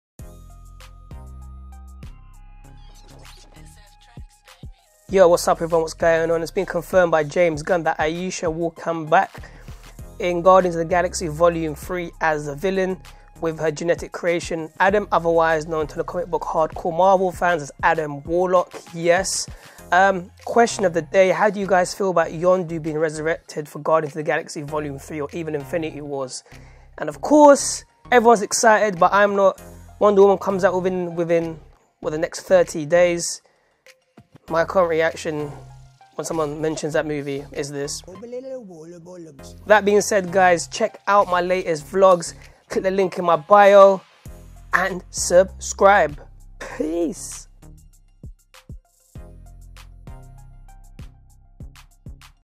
would you like to see .... in GOTG VOL 3 ? !!!
I would so love to see in vol 3! lol it would be so cool, Guardians Of The Galaxy is such a cool amazing movie! I so love Baby Groot